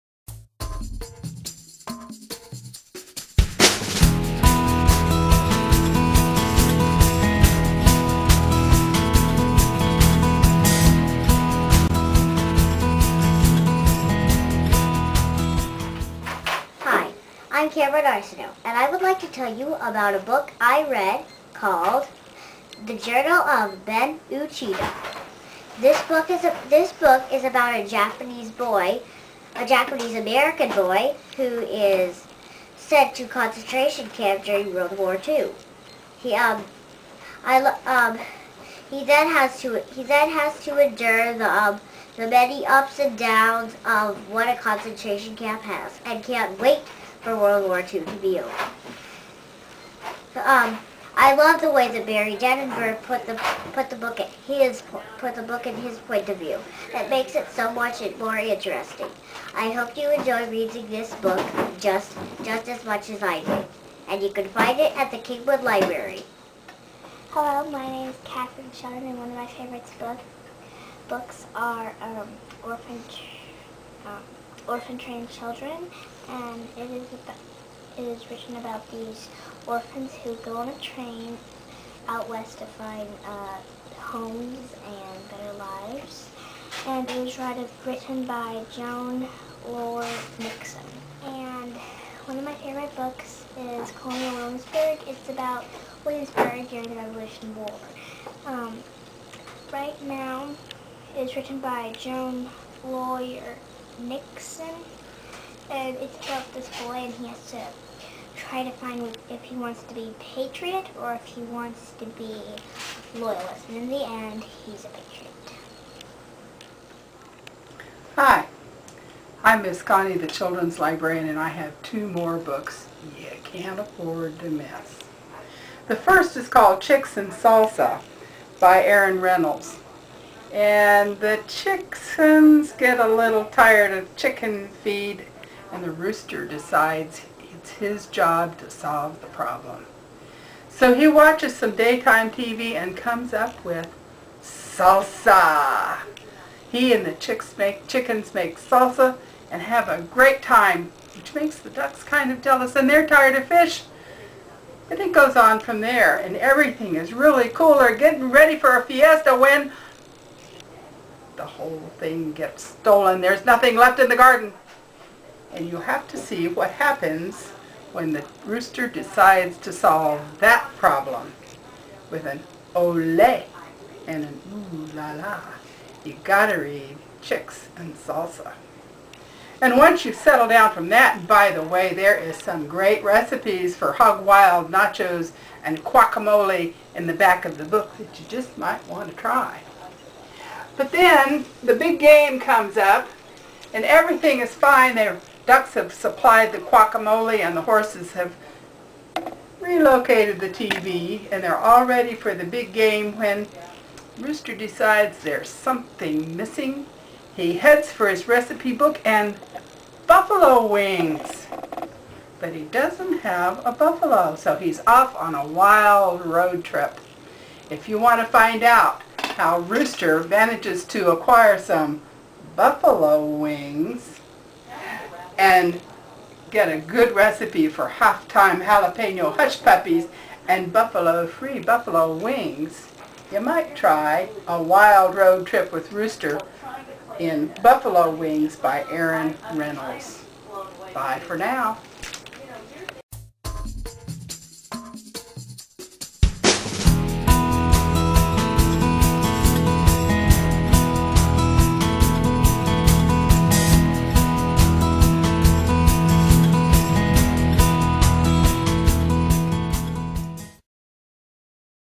Check it Out for Kids
Presented by Kingwood Branch Library, see what our patrons recommend for May 2009! This is just in time for the Summer Reading Program.